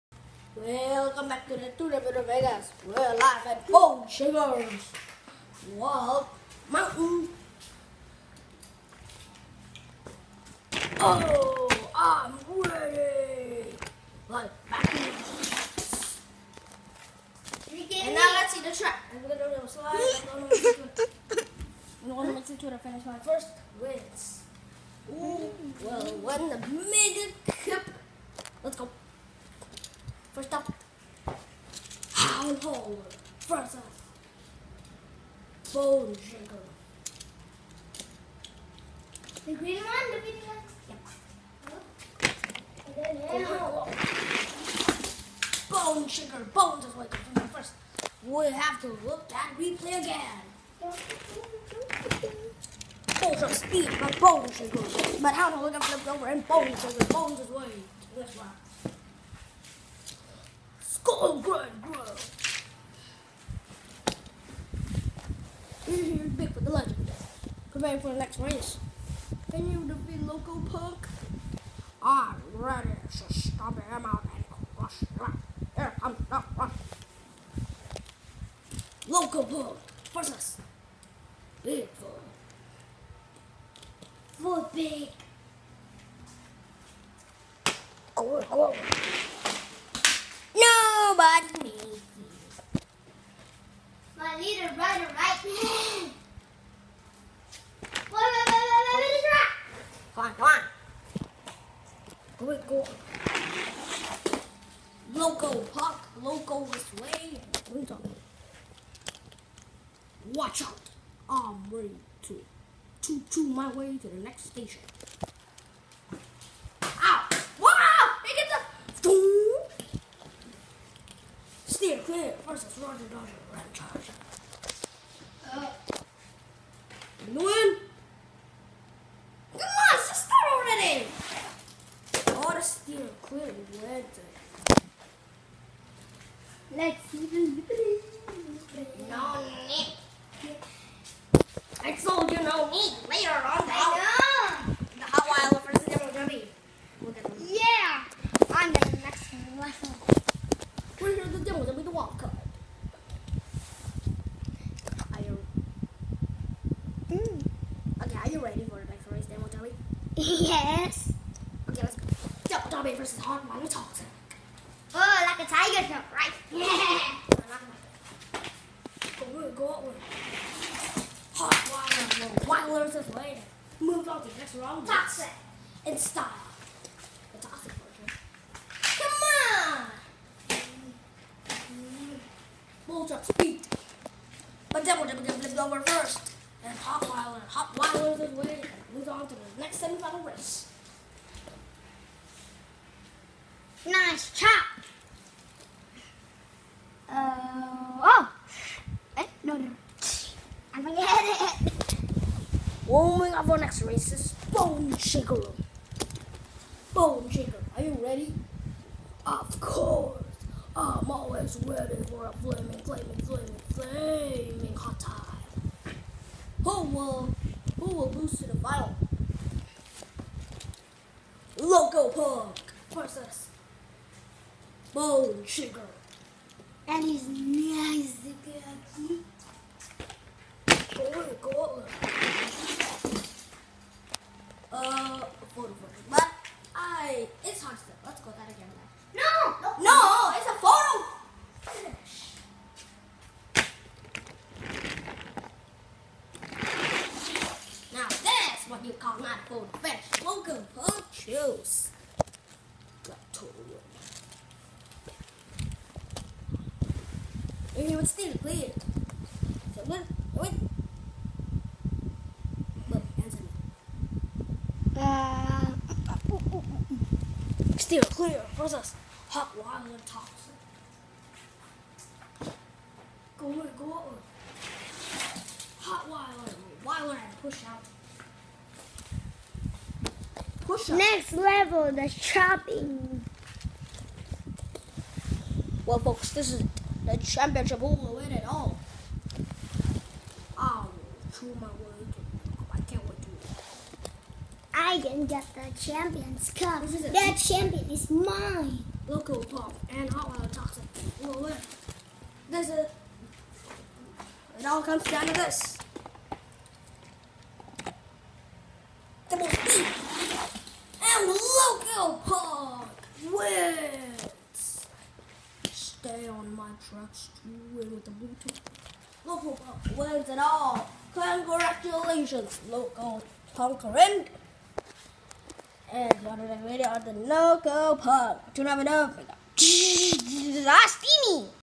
Bone shaker’s wild mountain
Today the monster trucks are racing at bone shaker’s wild mountain to see who will get the mega cup.